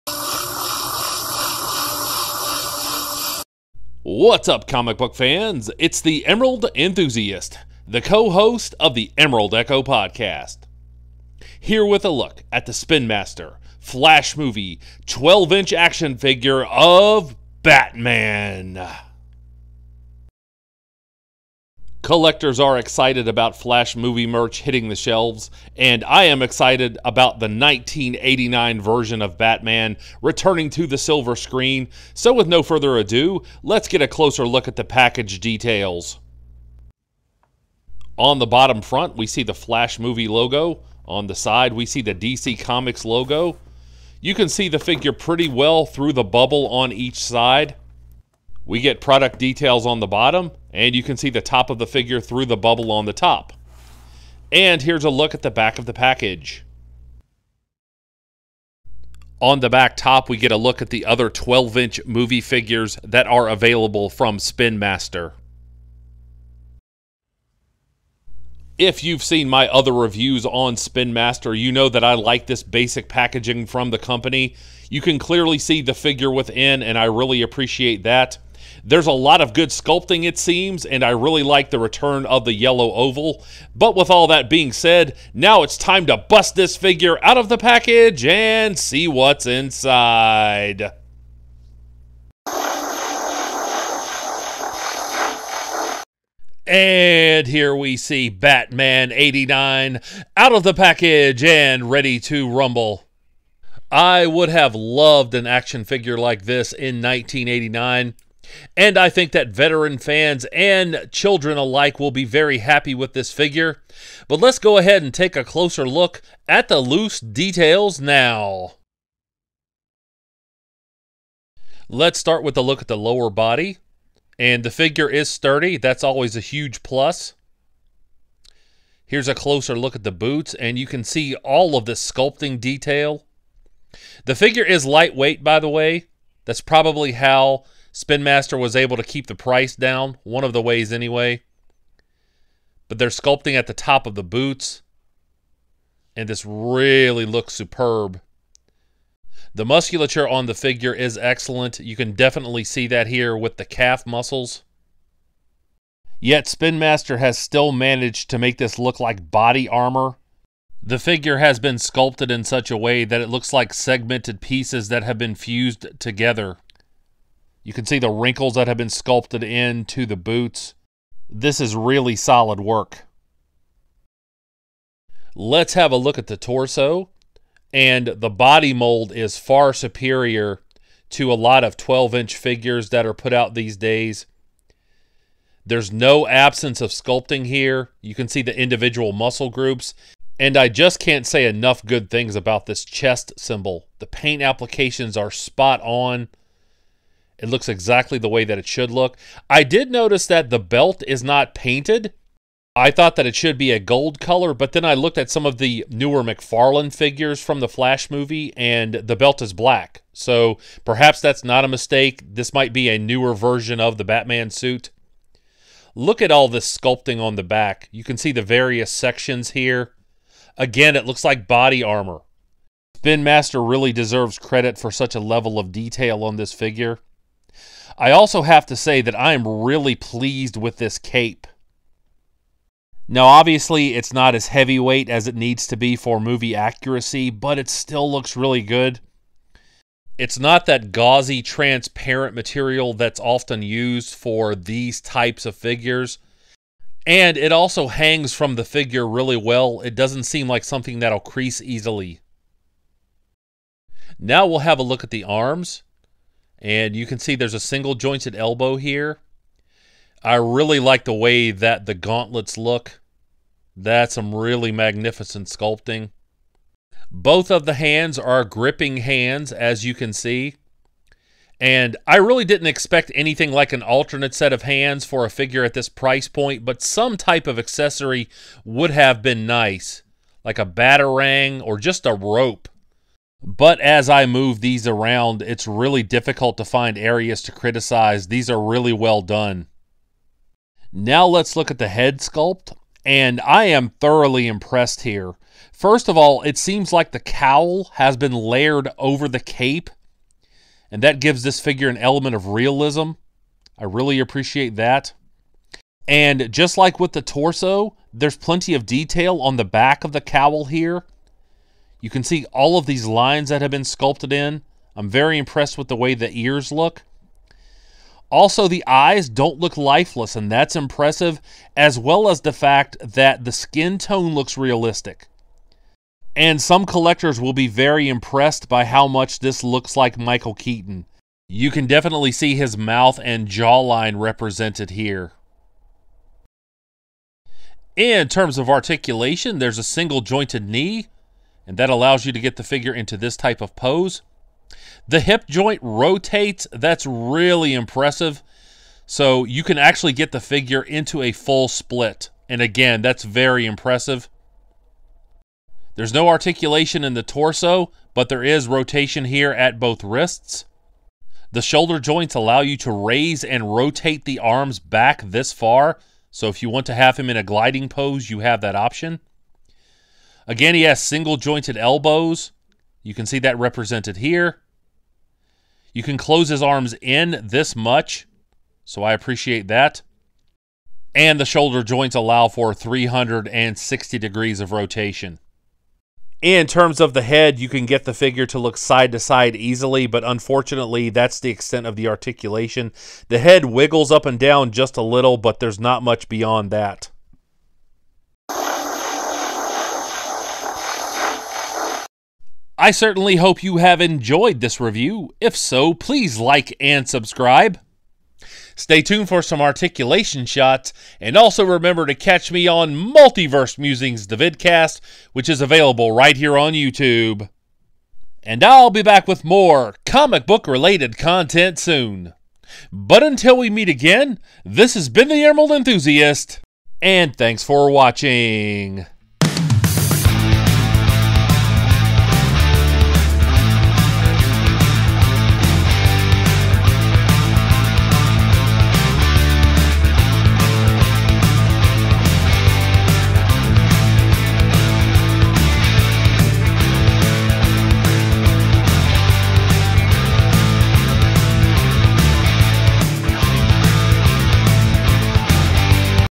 DC Comics Spin Master 12 inch The Flash Movie Michael Keaton Batman 1989 Full Action Figure Review
A review of this Flash Movie action figure from Spin Master.